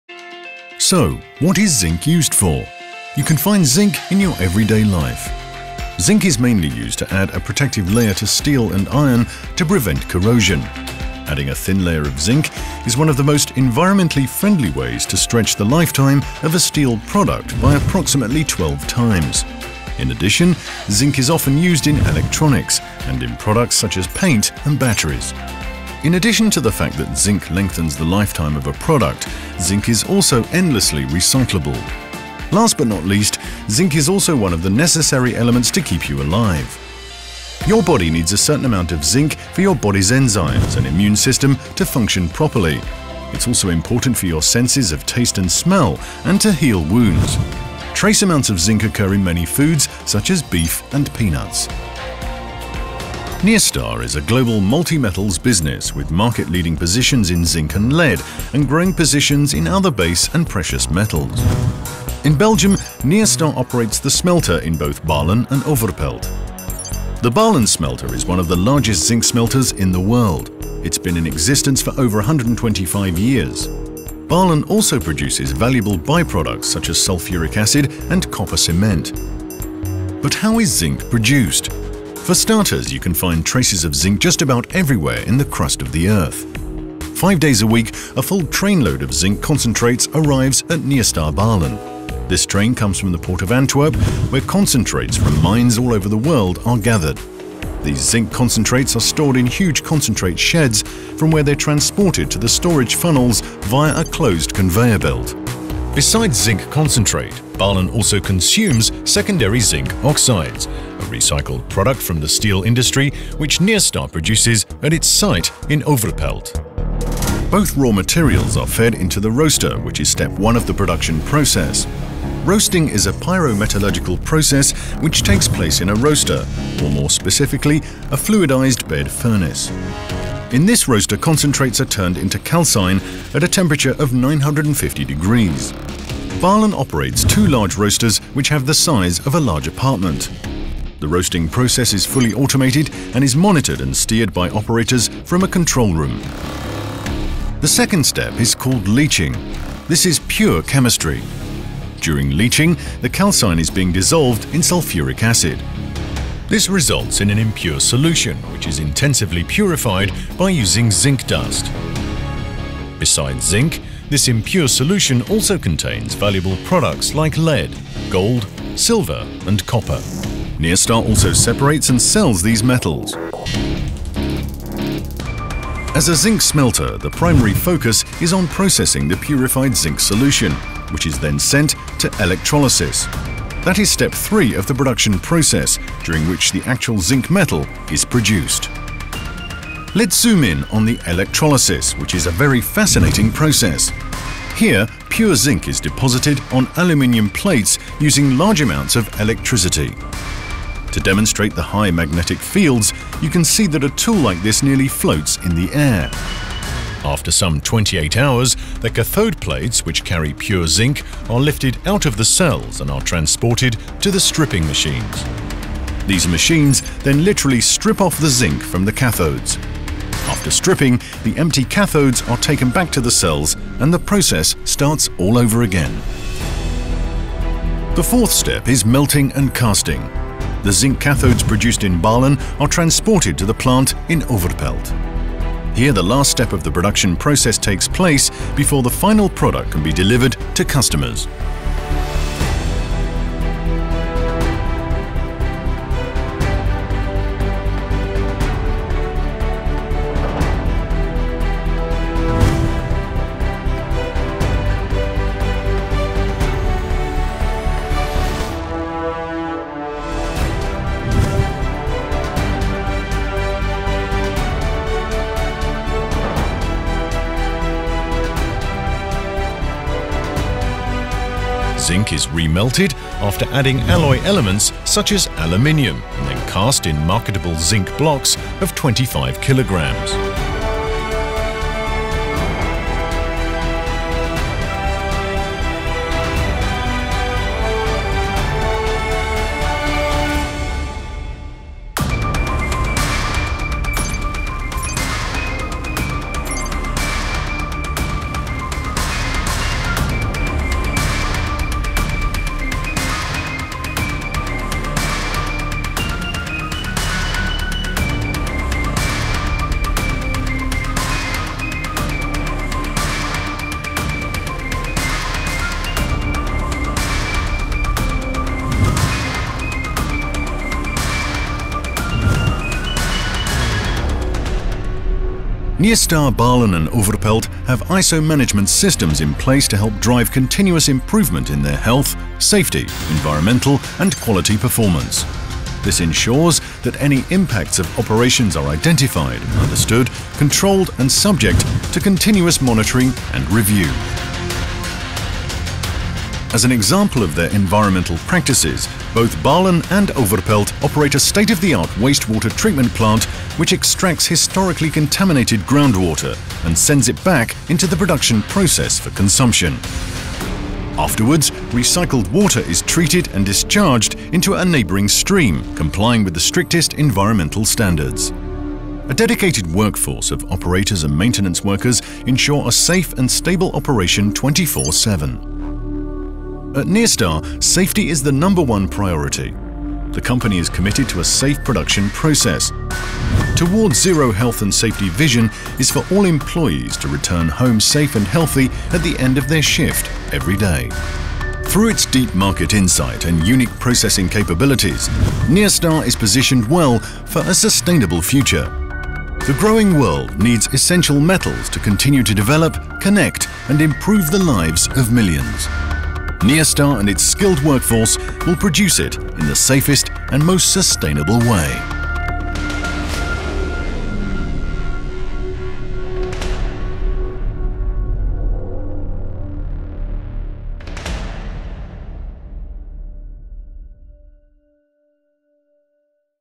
Nyrstar in Balen/Pelt (Ultra HD 4K)
In Belgium, Nyrstar operates one of the largest zinc smelters in the world, with sites in Balen and Pelt. The smelter is part of the Nyrstar Group, a global multi-metals business, with market leading positions in zinc and lead, and growing positions in other base and precious metals. This video explains how zinc at Nyrstar in Belgium is produced, in the safest and most sustainable way, and how it improves the lives of millions.